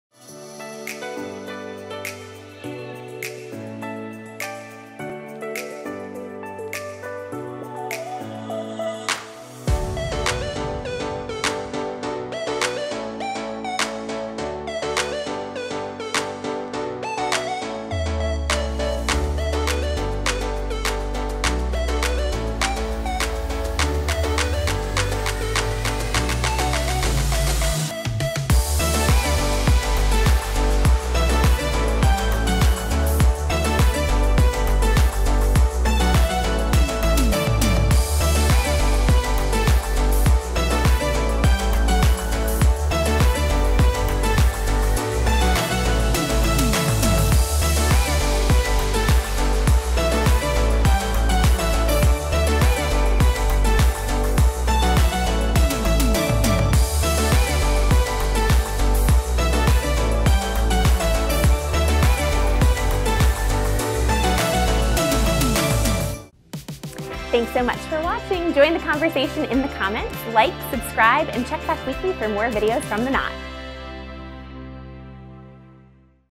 The Best Pool and Beach Themed Bach Party Essentials | The Knot
Here are the must have items from The Knot Shop to throw the best pool or beach themed bach party for your entire wedding crew.

Shop all the items featured in this video below.